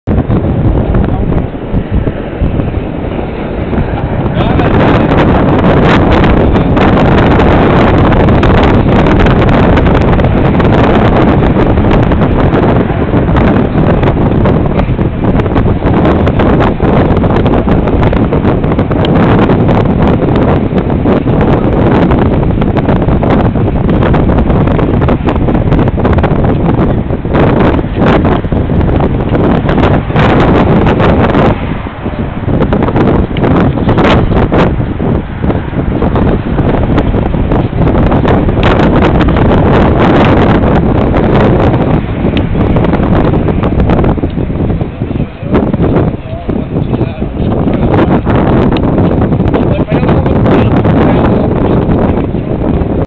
Eiffel Tower - view from the bridge
View of the Eiffel Tower seen frome the bridge on the river Seine in Paris, 2011